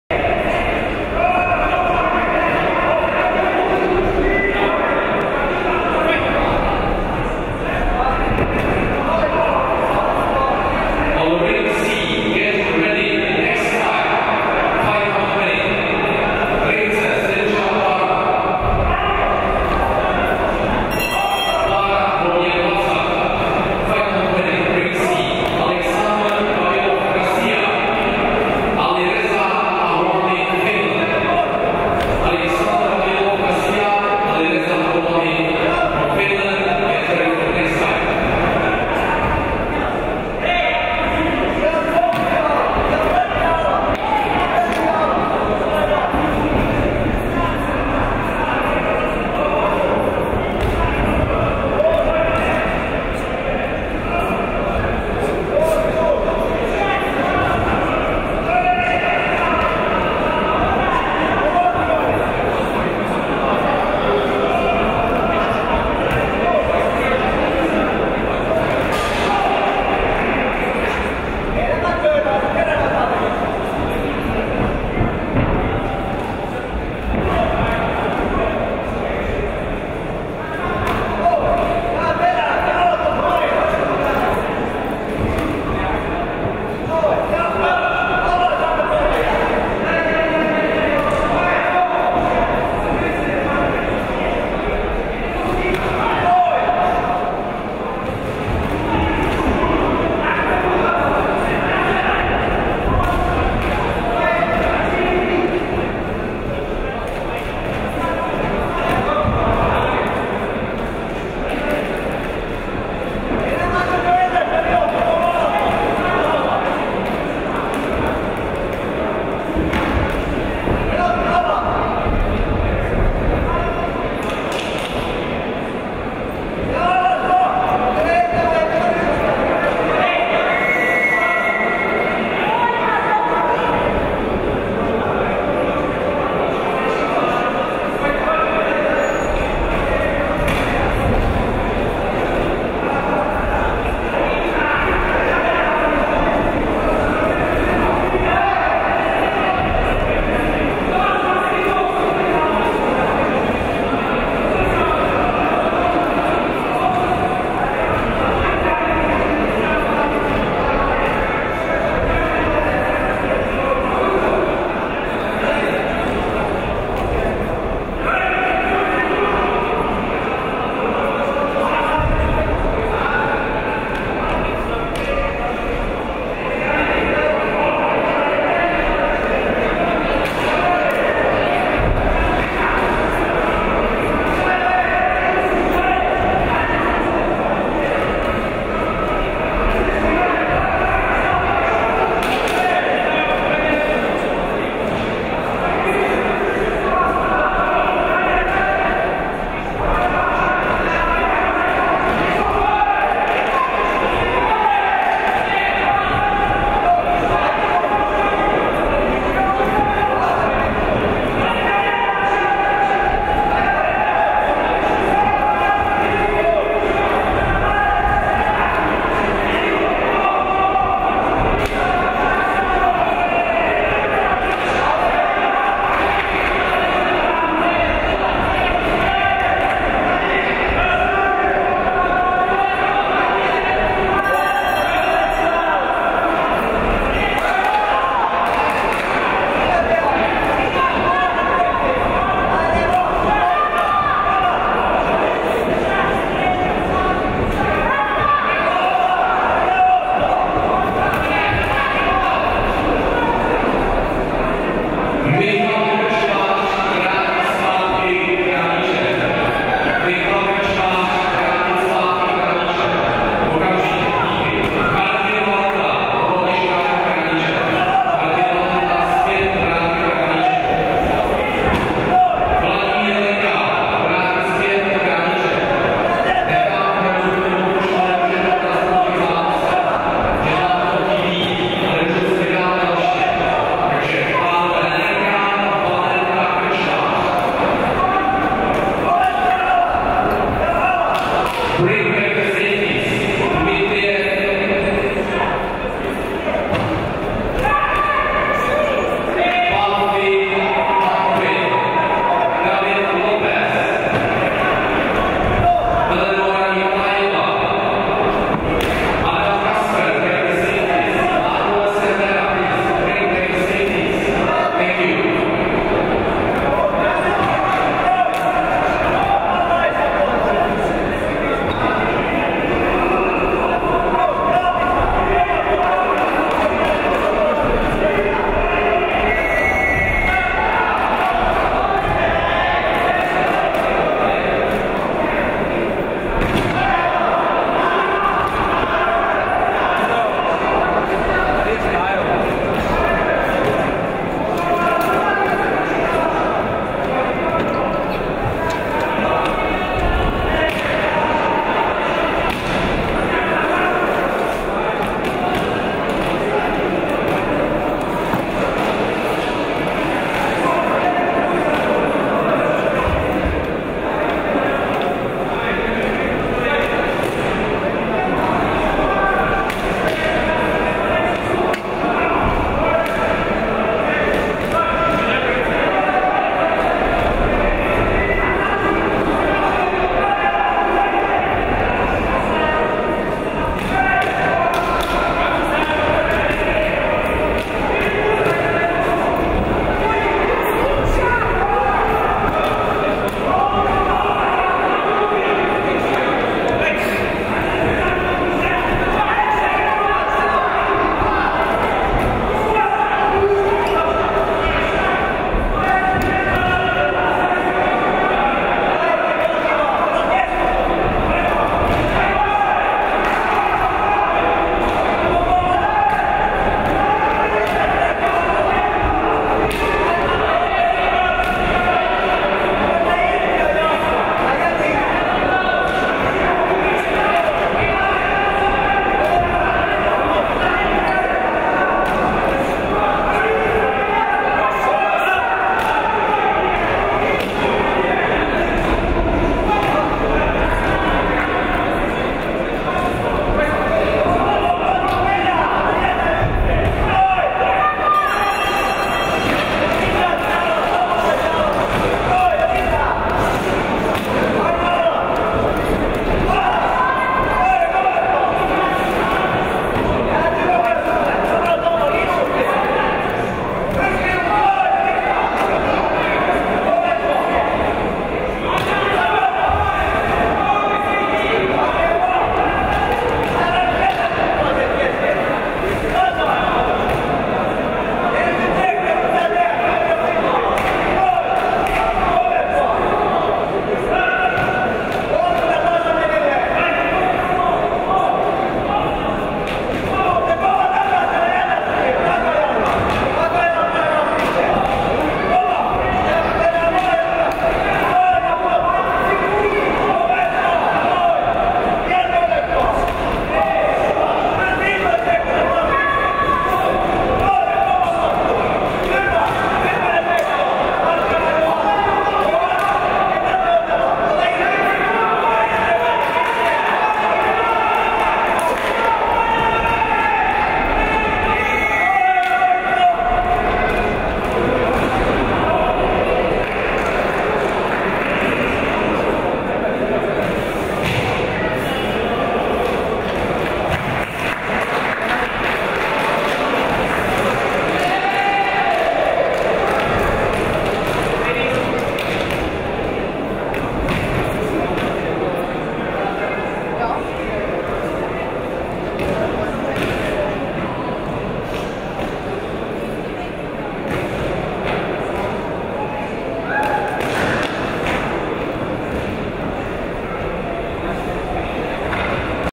WAKO K-1 WGP 2019 | OSETSKA - HARTIKAINEN
Final 1/4 S F -56 kg | static camera
OSETSKA ANTONINA (UKR) - HARTIKAINEN HANNA-RIIKKA (FIN)